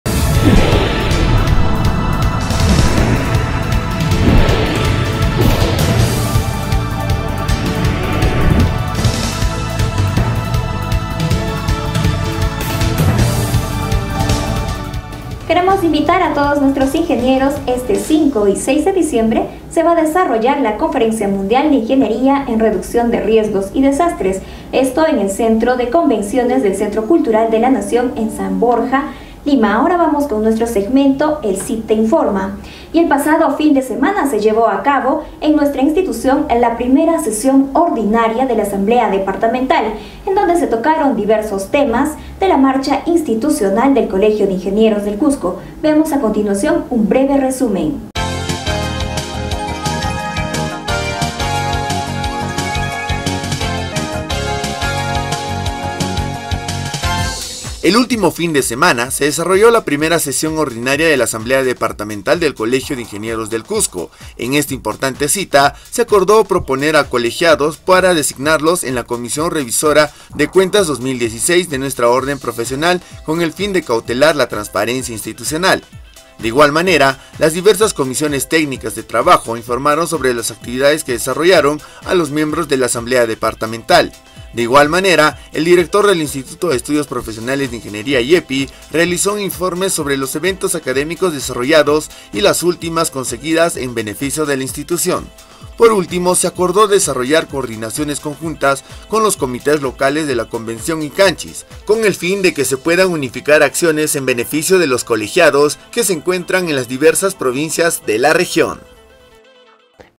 SESIÓN DE ASAMBLEA DEPARTAMENTAL.
LAHORADELINGENIERO (Domingo, 30 de octubre del 2016.)
El pasado fin de semana se llevó a cabo en nuestra institución, la Primera Sesión Ordinaria de la Asamblea Departamental en donde se tocaron diversos temas de la marcha institucional del Colegio de Ingenieros del Cusco.

CONDUCCIÓN:
- Kristian Huallpamaita Cárdenas
- María Isabel Hurtado Góngora

PRODUCCIÓN AUDIOVISUAL:
Elio Condorhuamán Chalco